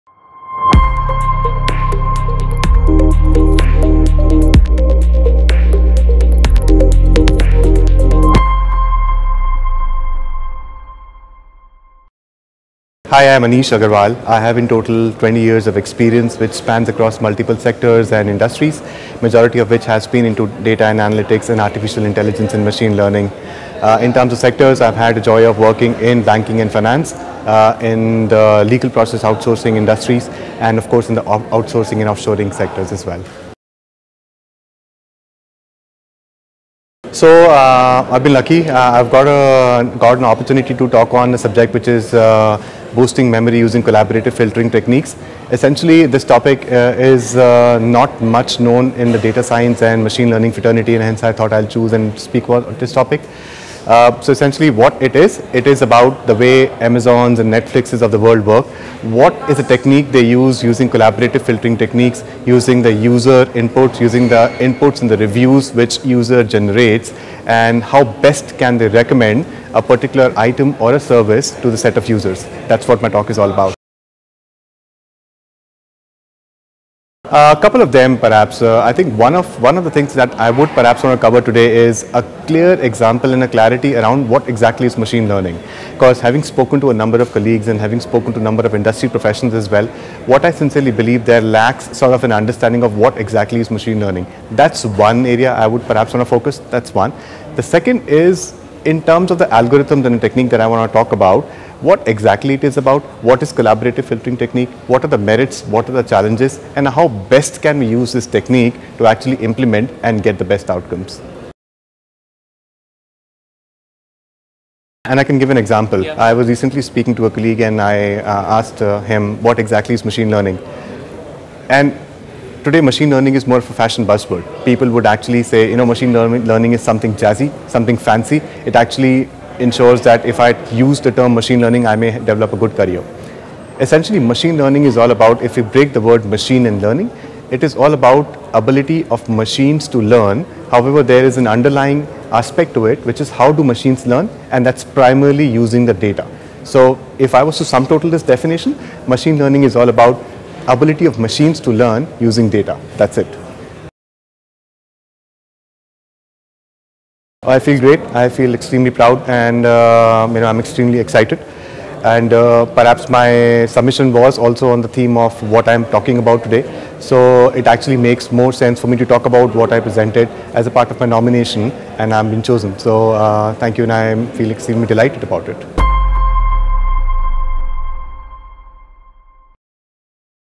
Anish Agarwal Director – Data & Analytics at RBS India [MLDS2020]
Machine Learning Developers Summit 2020
22-23rd Jan | Bangalore
30-31st Jan | Hyderabad

Recognised amongst Top 100 influential Leaders in Asian Analytics Industry, Anish is a professional with over 18 years of experience in multi-national organisations, with a proven ability to design and execute strategy and new technology adoption. He has held several positions of immense organisational importance with principal focus on delivering value through innovative use of data to drive decision making. Anish’s experience encompasses Data & Analytics, Project Management, Strategy, Financial Modelling and Artificial Intelligence. He has been on panel of a number of industry conferences and has also authored papers on various areas of Artificial Intelligence and advanced Data & Analytics. Anish is an alumnus of Harvard Business School and MIT Sloan Business School. He is also a member of advisory board of an AI start-up in India. Besides, Anish in on panel of a number of AI themed industry bodies and a visiting guest faculty in a number of tier 1 colleges and universities.